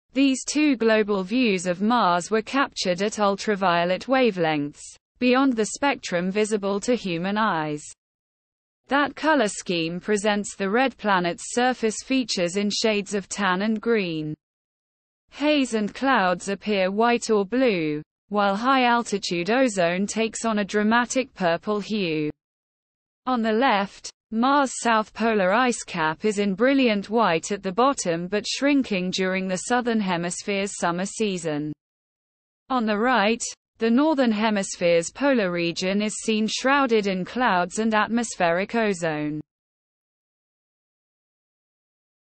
MAVEN's Ultraviolet Mars
These two global views of Mars were captured at ultraviolet wavelengths, beyond the spectrum visible to human eyes. Recorded by the MAVEN spacecraft's Imaging Ultraviolet Spectrograph instrument in July 2022 (left) and January 2023, three otherwise invisible ultraviolet bands are mapped into red, green, and blue colors. That color scheme presents the Red Planet's surface features in shades of tan and green. Haze and clouds appear white or blue, while high altitude ozone takes on a dramatic purple hue. On the left, Mars' south polar ice cap is in brilliant white at the bottom but shrinking during the southern hemisphere's summer season. On the right, the northern hemisphere's polar region is seen shrouded in clouds and atmospheric ozone. Known to some as the Mars Atmosphere and Volatile EvolutioN spacecraft, MAVEN has been exploring Mars' tenuous upper atmosphere, ionosphere, and its interactions with the Sun and solar wind since 2014.